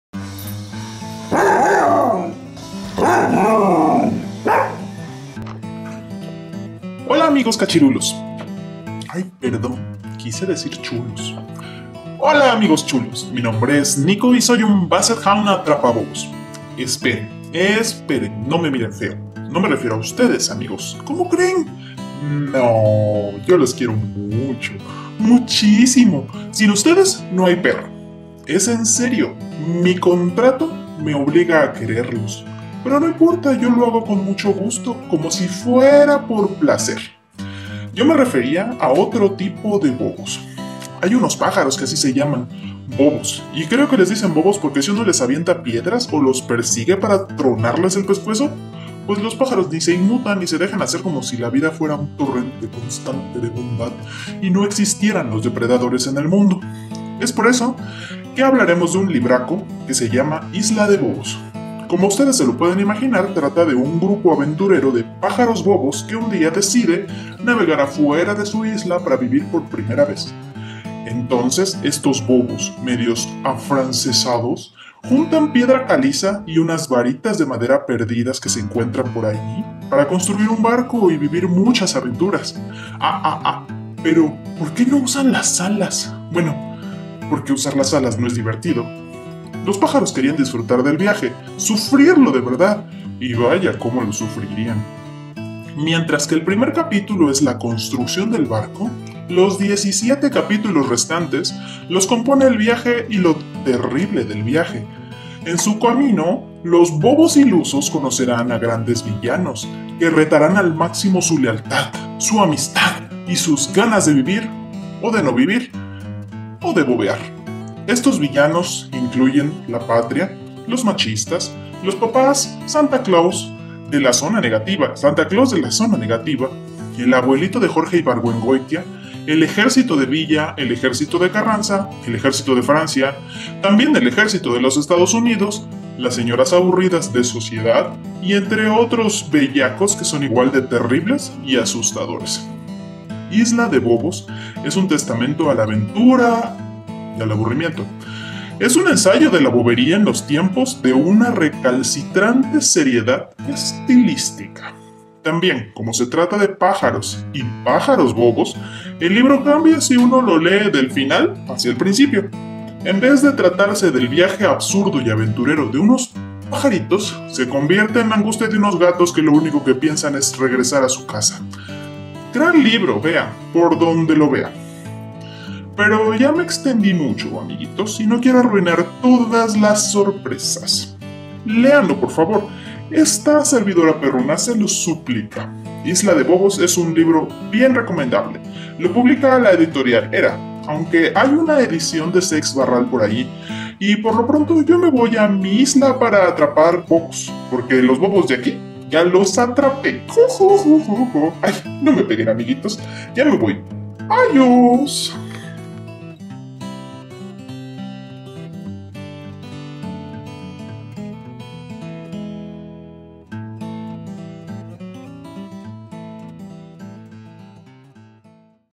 Isla de bobos
Mi nombre es Nico y soy un Basset Hound.

Todos dicen que los perros no pueden leer, esta semana yo me devoré Isla de bobos de Ana García Bergua. ¿Conoces este libro? ¿Sabes de qué trata la historia? Mira mi video y te lo cuento todo, o un poquito y luego me hago bolas. Ponme en los comentarios si conoces a esta autora o algún otro autor mexicano.

¡Nos vemos a la próxima!
AYOS AMIGOS

· ¡Encuéntrame! ·